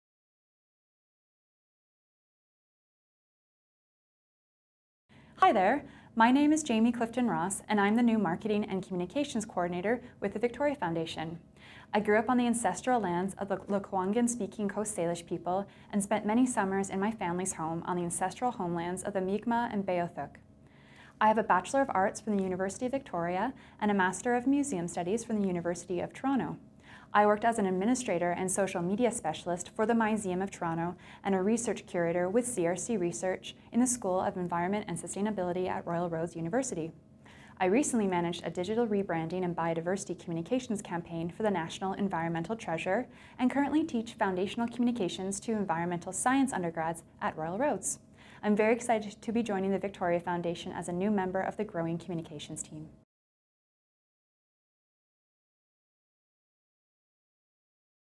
Meet Our Team: Jaime Clifton-Ross
The Victoria Foundation would like to introduce you to our newest staff member, Marketing and Communications Coordinator Jaime Clifton-Ross. Watch the video to learn more.